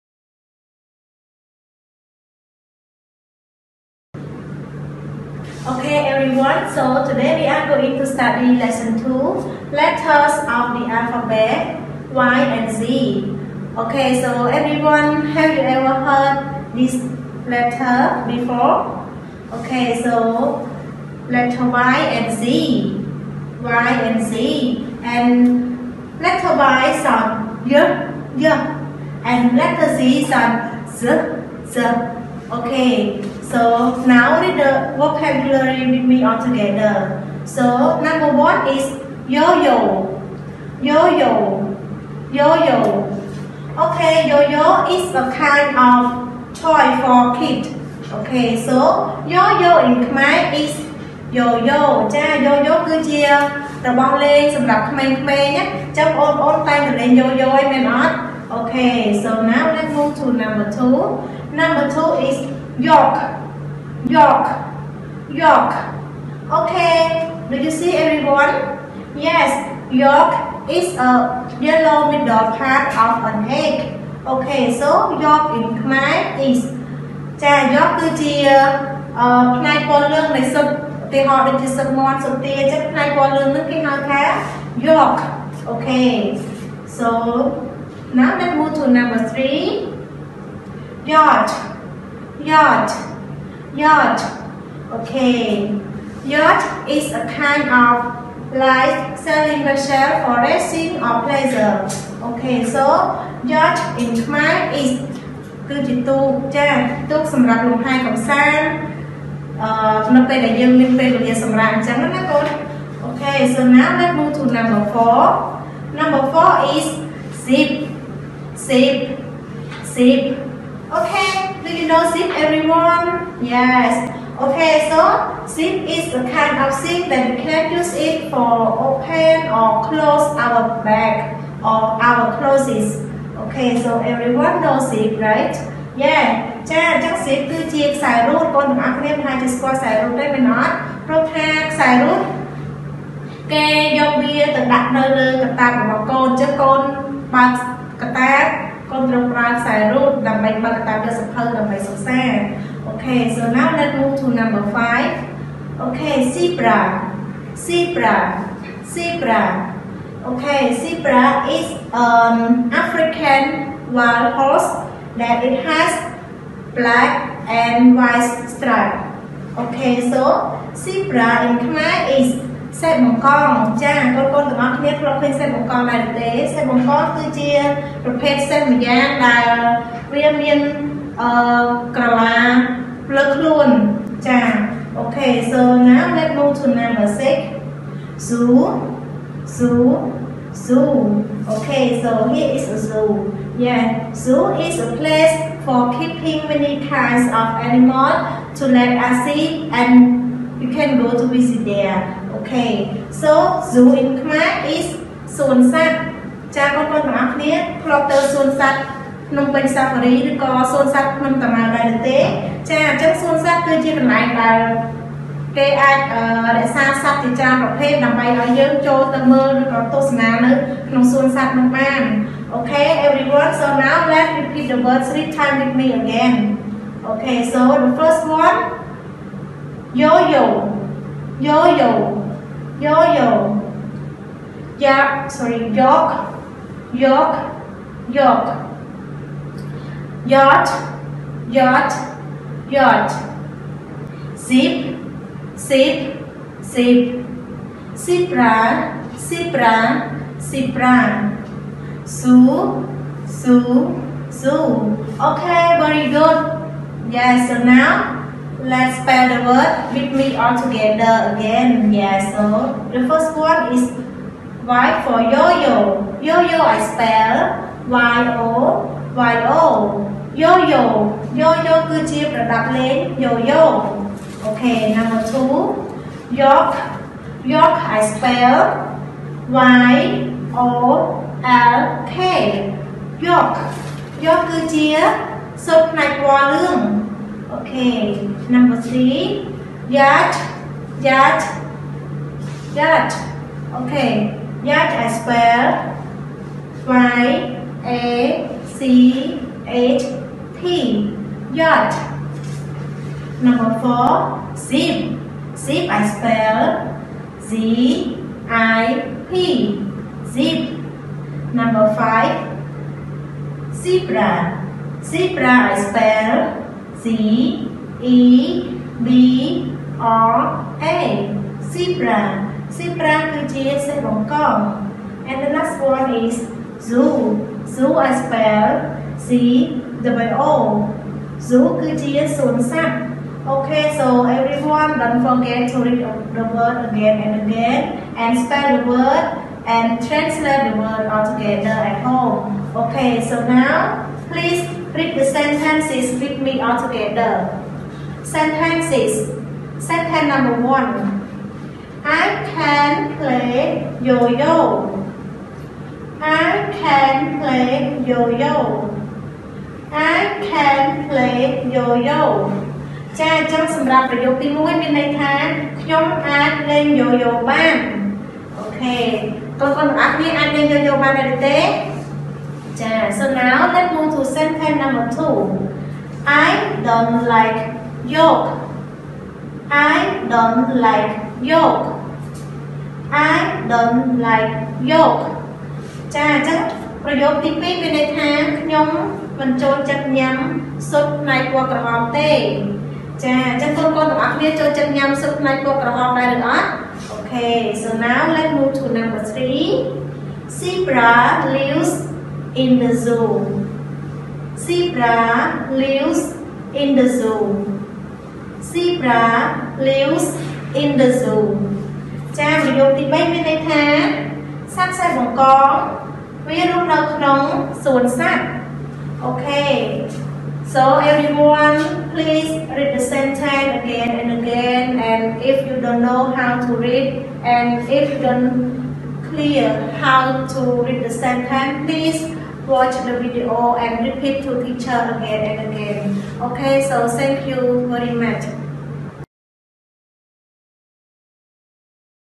Letters of Alphabets Yy zz Section 2
This video teach about Lesson 2 preschool 1 Letters of Alphabets Yy zz Section 2.
Best wish good luck.
The more you learn the more you Know !